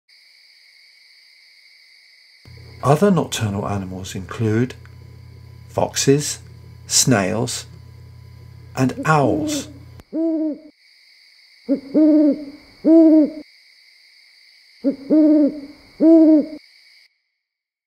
Other Nocturnal Animals - short
An introduction to nocturnal animals for younger children.
With thanks for the use of photos from the following for use of their Unsplash images: Shallow Focus Fox by  Jiri Sifalda and  Owl by Jon Del Rivero.